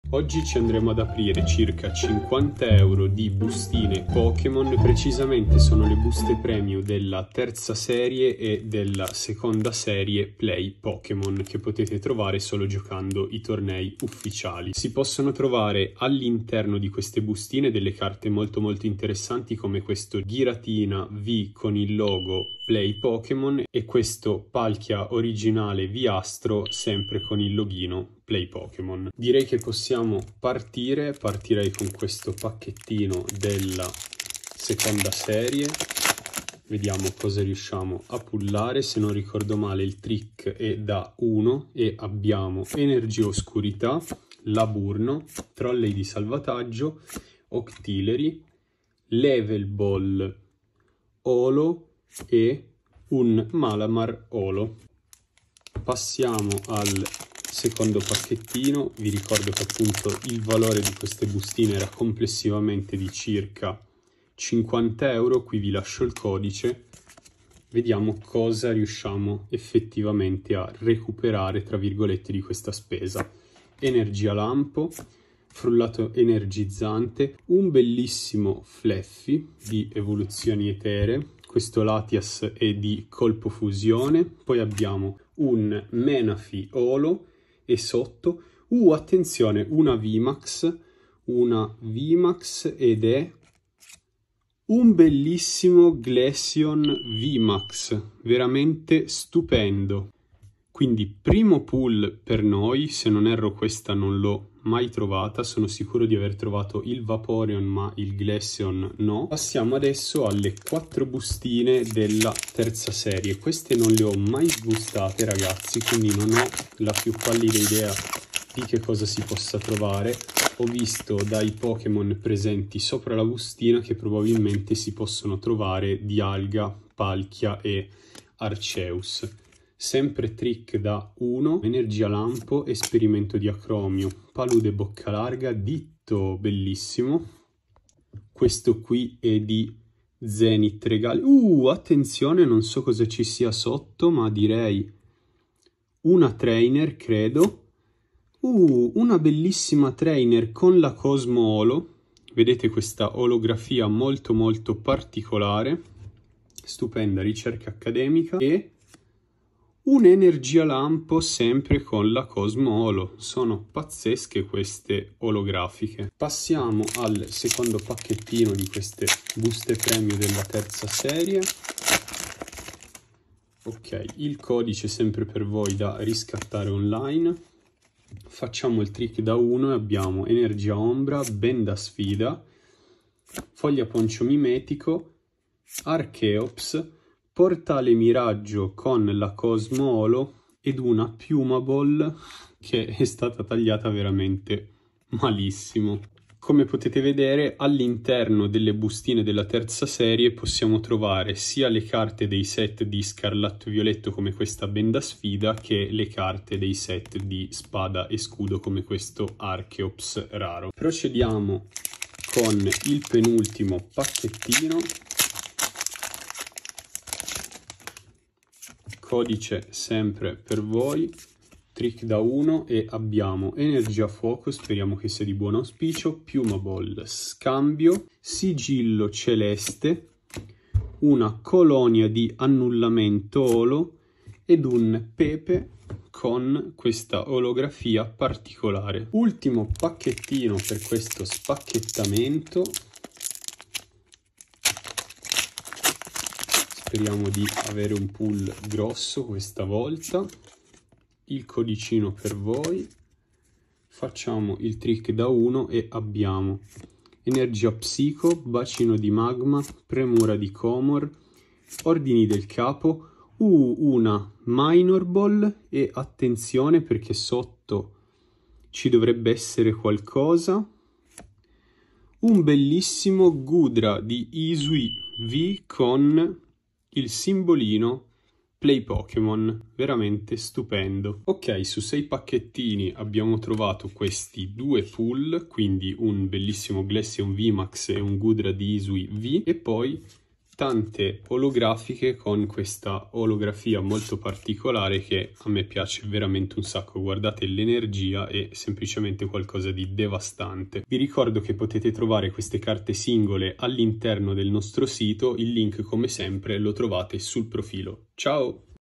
APRIAMO €50 DI PACCHETTI POKÉMON DELLA SERIE PLAY!
Vediamo cosa riusciamo a trovare in €50 di bustine della Serie Play! Pokémon